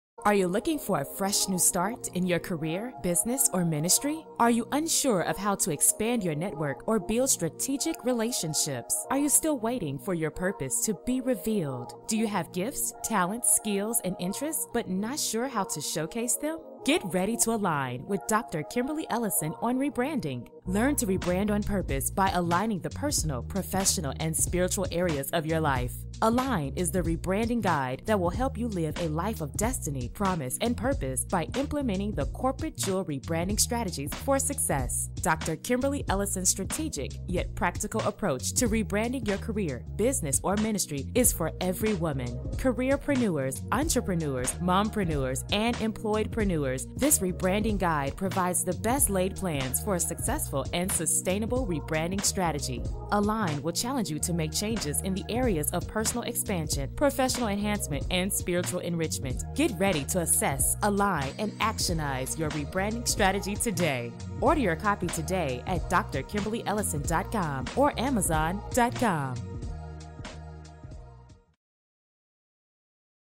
ALIGN by Dr. Kimberly Ellison
Are you looking for a fresh new start in your career or business?
Are you unsure of how to expand your network or build strategic relationships?
Are you still waiting for  your purpose to be revealed?
Do you have spiritual gifts, talents, skills and interests but not sure how to showcase them?

Get ready to ALIGN your rebranding strategy for success with Dr. Kimberly Ellison's new book. ALIGN will take you on a journey of discovery and a pathway to purpose through Dr. Kimberly's Corporate Jewel Rebranding Strategies for Success. Learn to use what's already in you to live a life of destiny, promise and purpose.